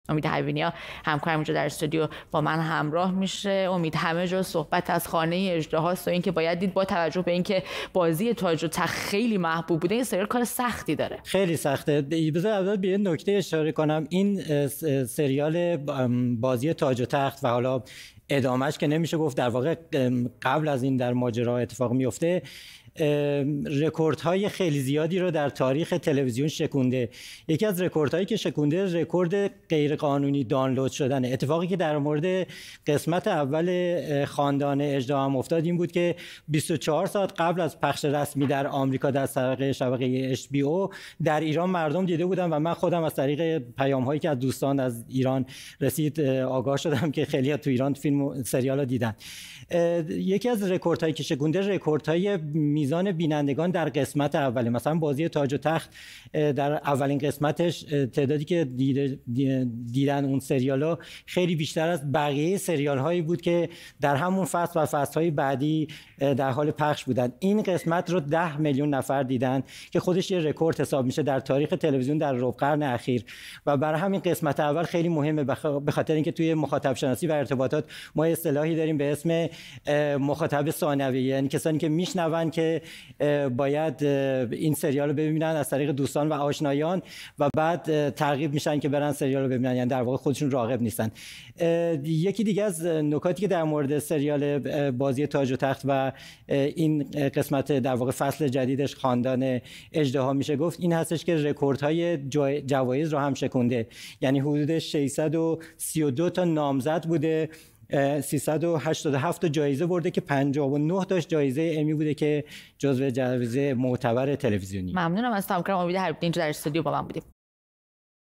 آغاز پخش سریال خاندان اژدها
آغاز پخش سریال خاندان اژدها پس از موفقیت‌های سریال «بازی تاج و تخت»، در گفت‌وگو با امید حبیبی‌نیا، ایران اینترنشنال